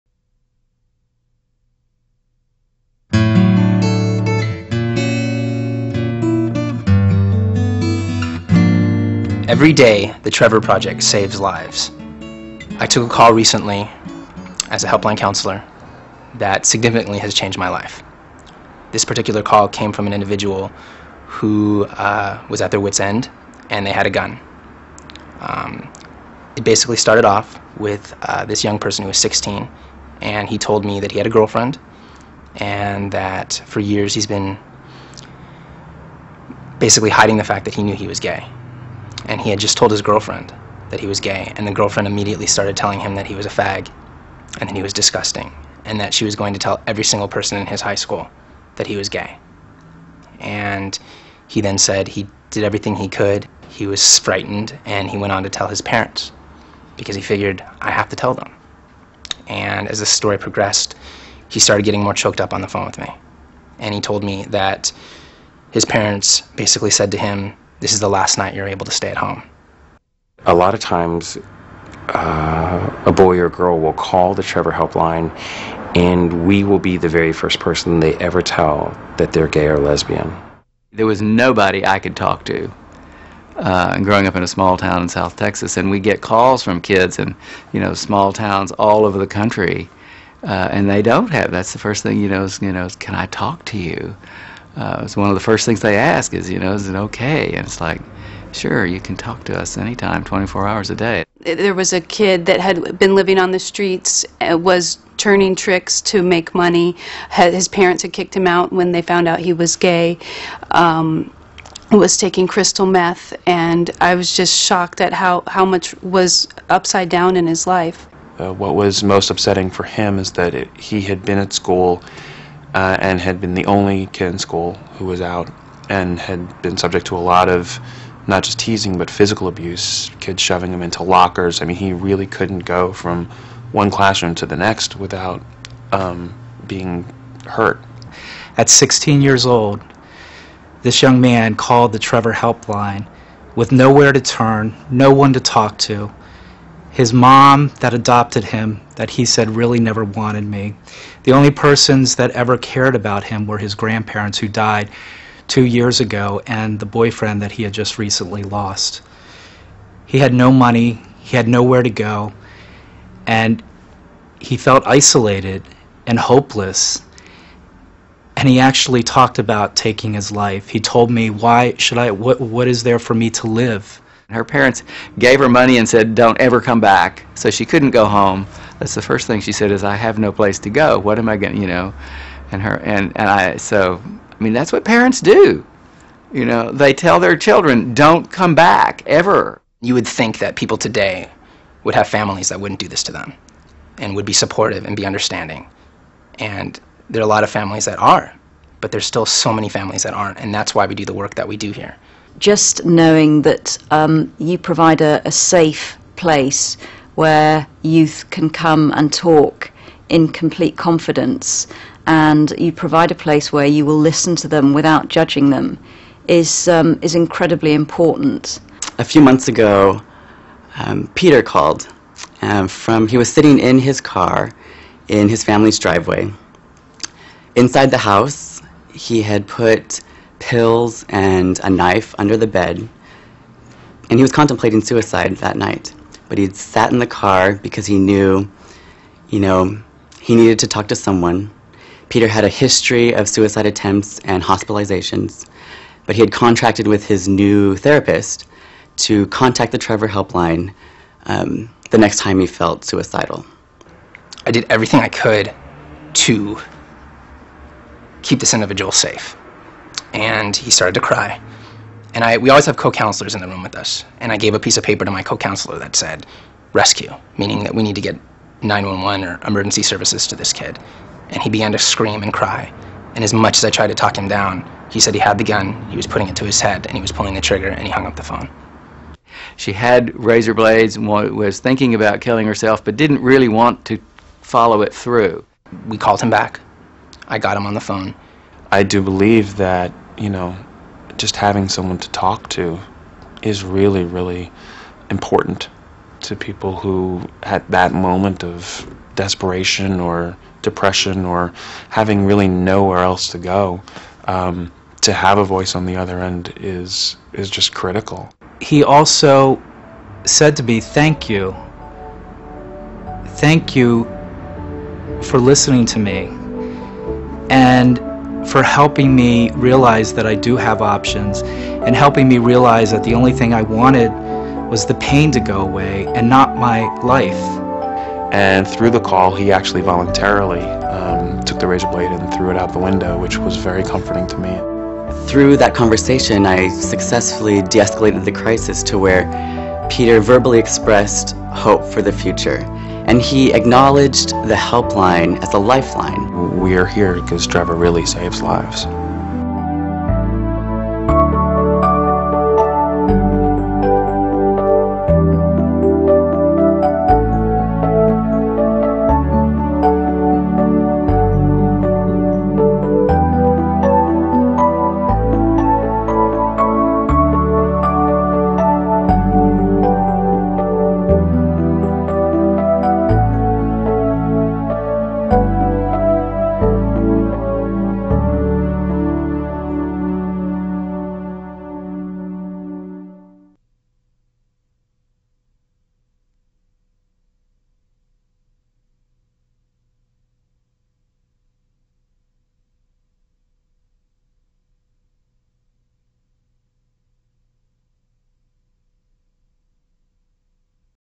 Trevor Project - Stories from the Frontline
Real helpline counselors who answer the phones of the Trevor Helpline recount calls they've received from LGBTQ youth across the country.

Call the Trevor Helpline at 866.4.U.TREVOR for someone to talk with 24/7.